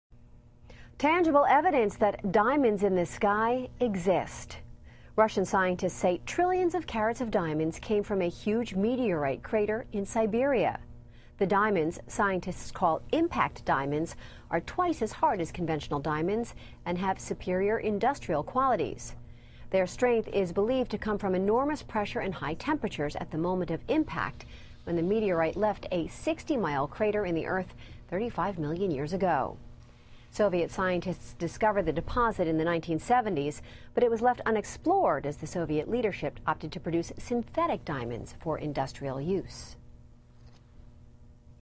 Diamonds fall from the sky in Siberia
Russian scientists say trillions of carats of diamonds were discovered in a meteorite crater in Siberia that dates back 35 million years